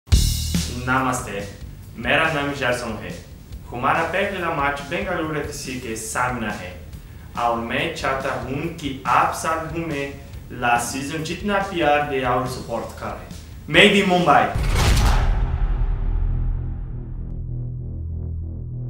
Mumbai City FC | Gerson Vieira - Message in Hindi
Samba Boy in desi style! 😎🇧🇷🇮🇳

Looks like ‪Gerson Vieira has picked up a bit of Hindi & here's a special message from him for all our fans ahead of tomorrow's game against Bengaluru FC! 👏‬